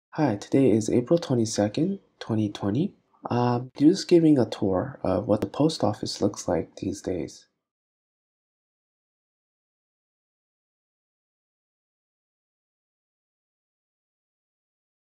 Post Office visit April 2020
Giving a tour of the post office in April 2020 during the coronavirus. This video shows how the post office employees are protecting themselves while working during the coronavirus.